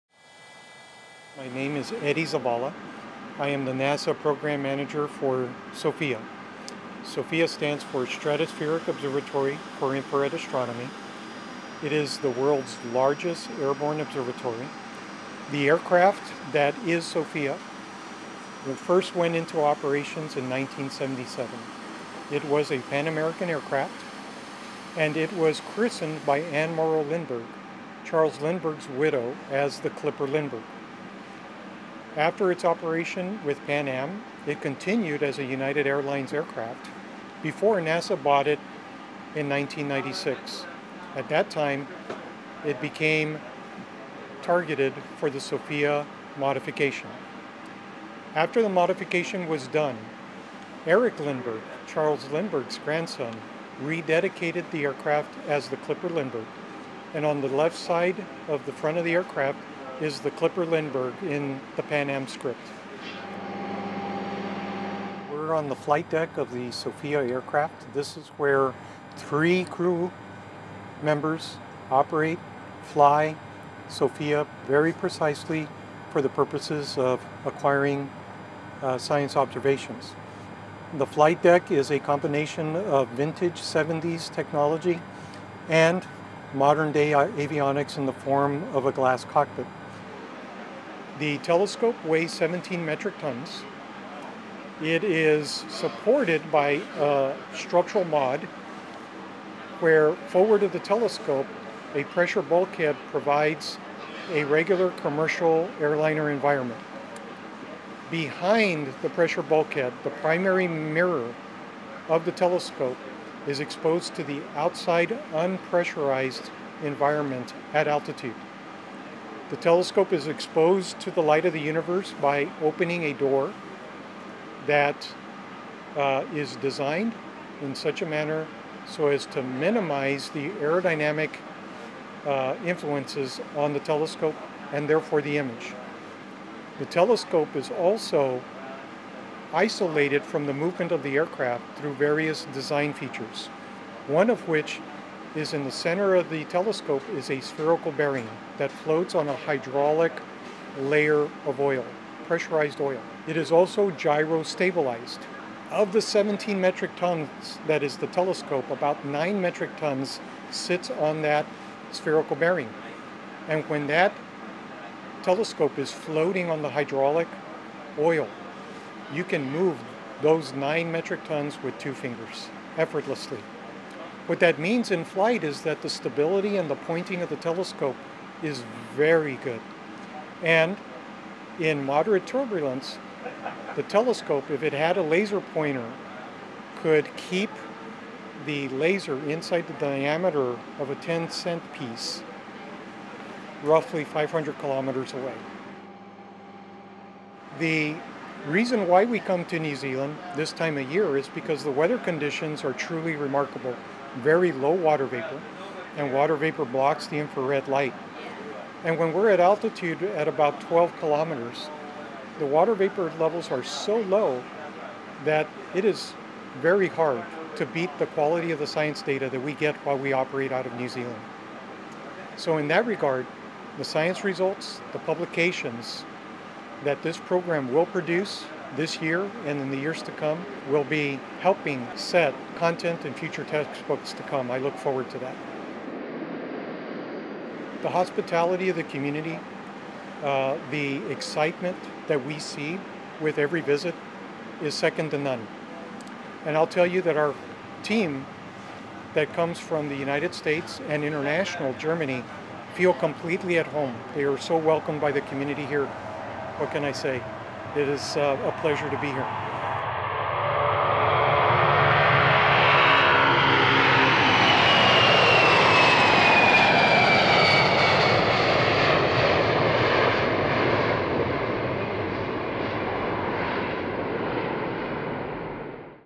A sneak peek inside NASA’s Stratospheric Observatory for Infrared Astronomy, SOFIA
Christchurch Airport is proud to host NASA’s Stratospheric Observatory for Infrared Astronomy, known as SOFIA, for two months’ observations in the Southern Hemisphere. For operational reasons, it is not possible to open SOFIA to the public, so we have made a short video to show you through the famous airborne observatory.